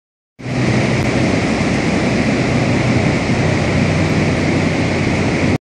500 cow dairy in Wisconsin with 72-inch exhaust fans
a view of the exhaust fans running during testing stages.